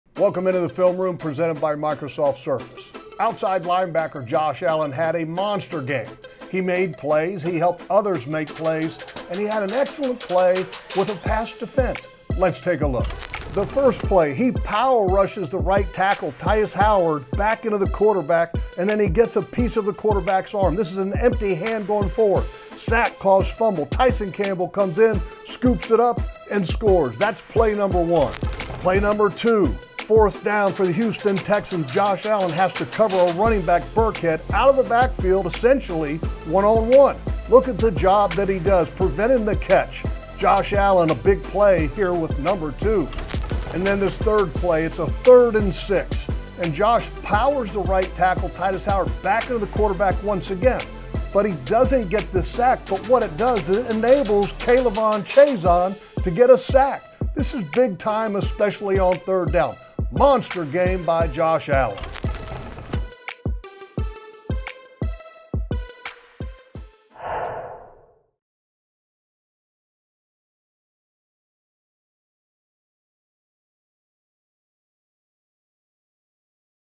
Josh Allen causing disruption vs. Texans in Week 17 | Film Room | Jacksonville Jaguars
Jaguars Outside Linebacker Josh Allen caused problems for the Texans in coverage and behind the line of scrimmage in Week 18. Jeff Lageman breaks down a few plays from the game demonstrating the disruption he caused on Microsoft Film Room.